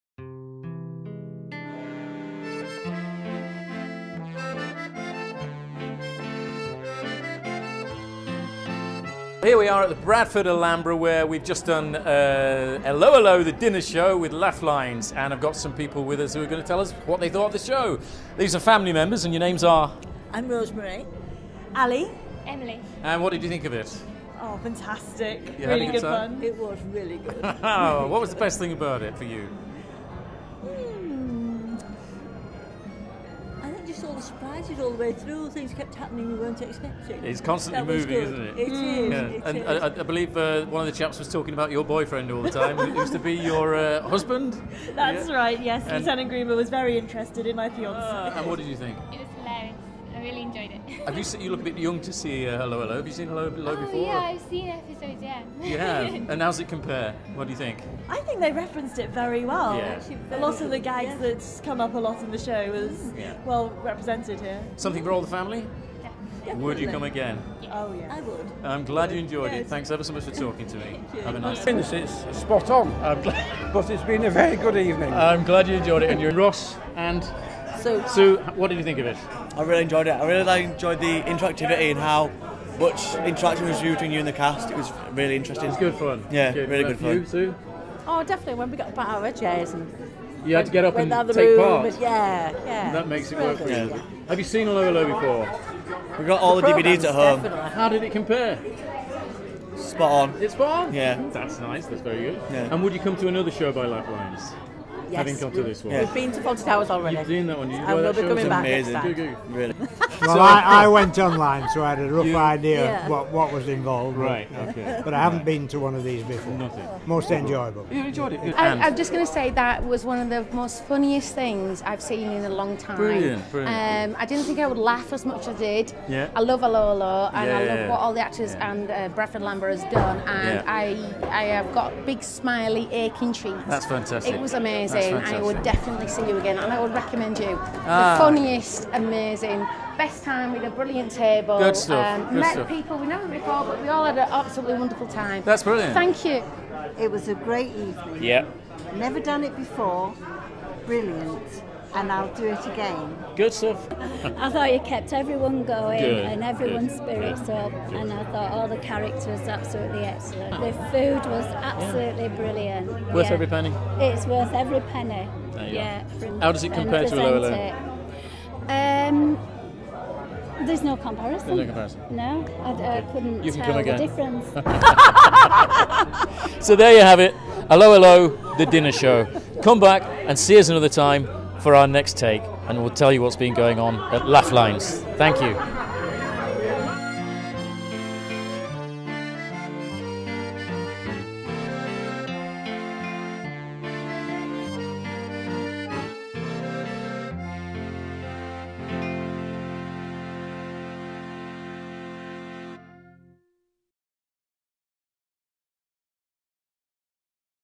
Allo Allo - Le Dinner Show! | Maritime Room, Cliffs Pavilion | Thu 6 & Fri 7 Feb
'Listen very carefully; I shall say this only once'

The hilarious 'Allo 'Allo -- Le Dinner Show is coming to The Cliffs Pavilion!

Enjoy a delicious three-course meal with half a bottle of wine per person in the famous Café Rene!
You will be transported back to the 1940's as Rene', his wife Edith and the loving waitress Yvette, perform this live comedy dinner theatre show, as if you were on set in the famous Café Rene'. Guests of honour attending tonight's dinner will be Head of Gestapo, Herr Flick & the very camp German officer Lieutenant Gruber.

The show promises an evening of side-splitting entertainment as well as a great meal, one of the most enjoyable dining experiences you will ever have!